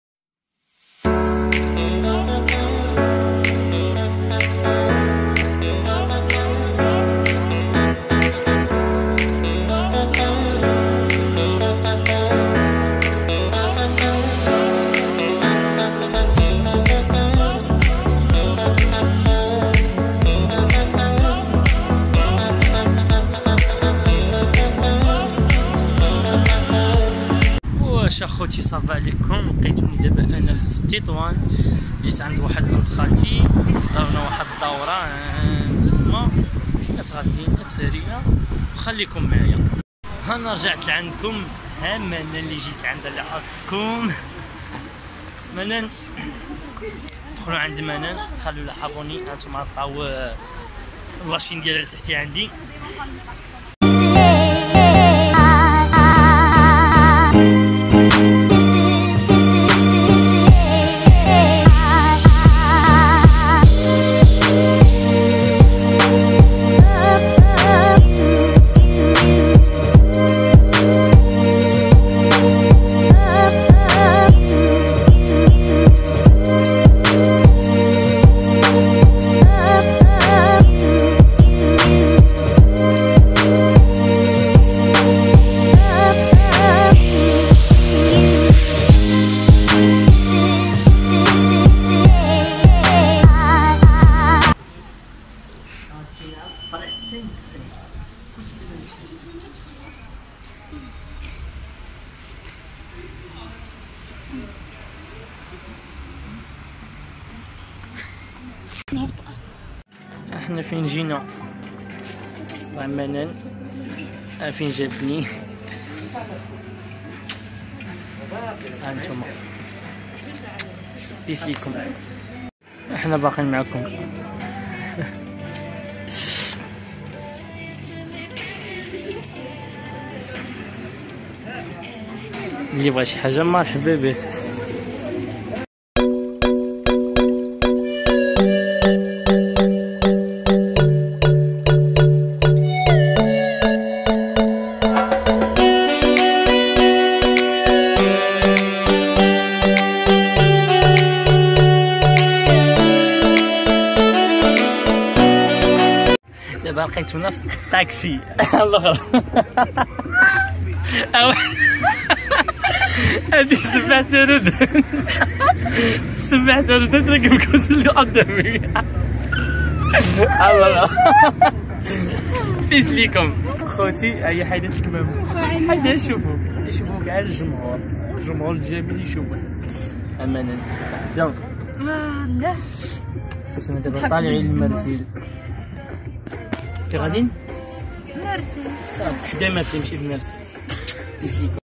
مشيت ل تطوان❤️ دازت احسن خرجة وأحسن Vlog
الخووت ها فلوك جديد بمدينة تطواان و فلوك نايض و أحسن خرجة ♥️♥️🇲🇦